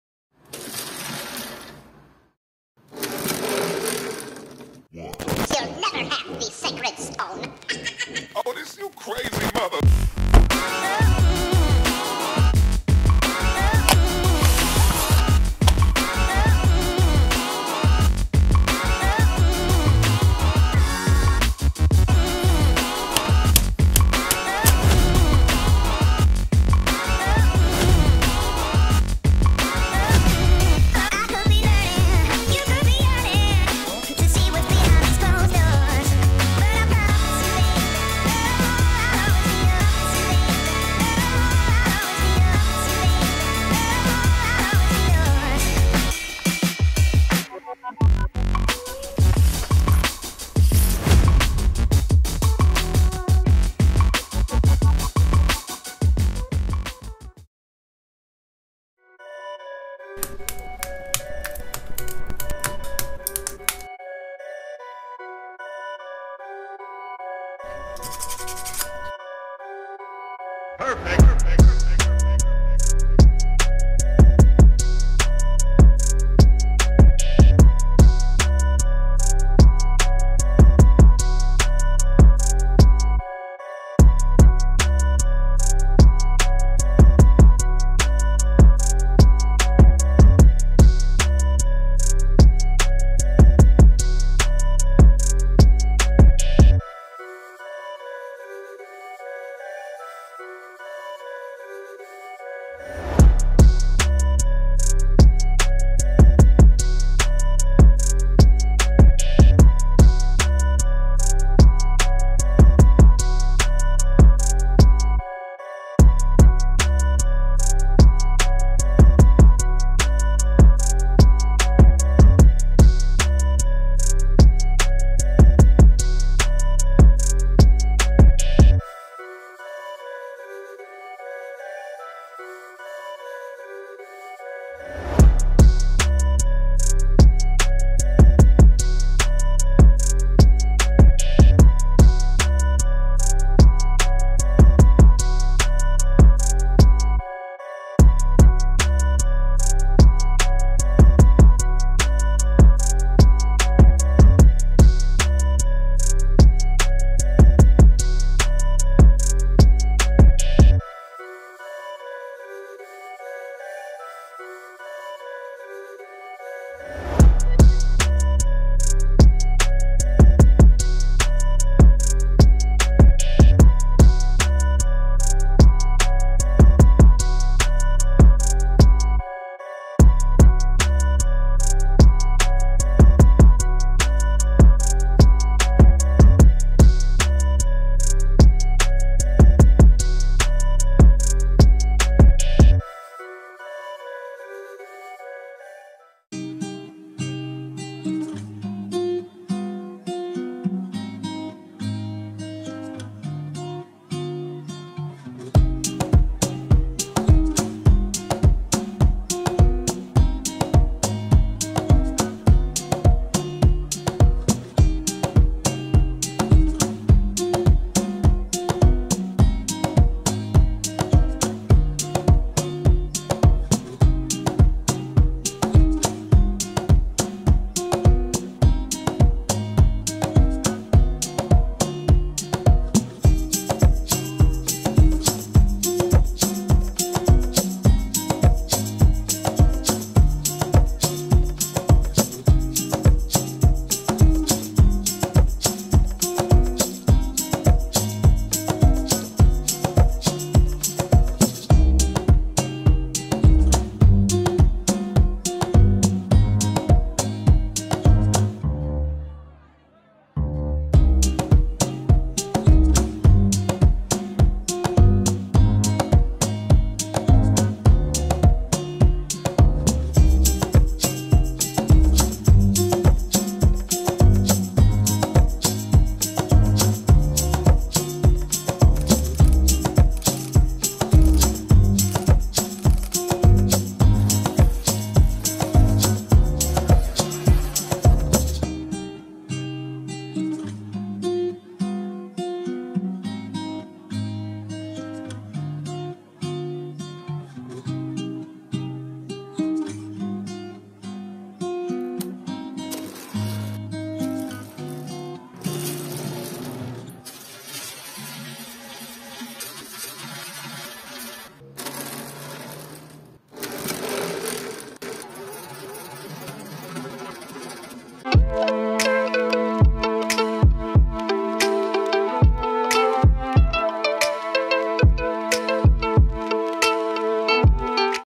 Assembly a Working Wooden TRAM LINE | Ugears 3D Puzzle | Speed Build & Review | ASMR
Wooden Magic Story with Train. You want to see it!
Wood Diorama -TRAM LINE- Ugears 3D Puzzle. Mechanical Town collection.